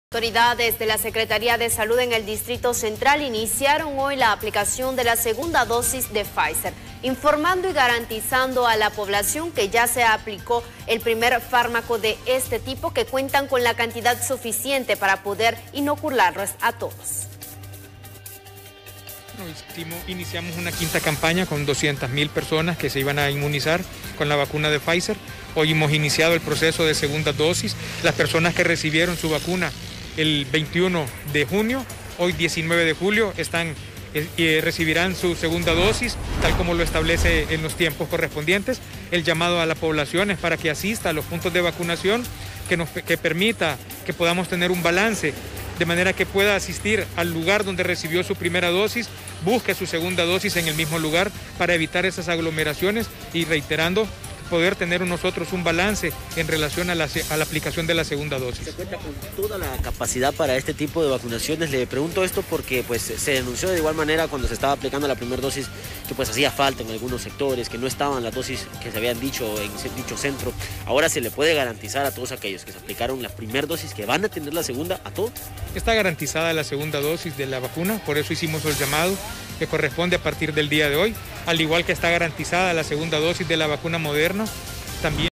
Hoy se inicia la aplicación de la segunda dosis de Pfizer
Las autoridades garantizan la segunda dosis a todos los que fueron inmunizados con este fármaco
Autoridades de la secretaria  de salud, expresan que hoy se inicia la aplicación de la segunda dosis de Pfizer, informando y garantizando a quiénes se aplicaron  la vacuna que cuentan con la cantidad  necesaria del inoculante.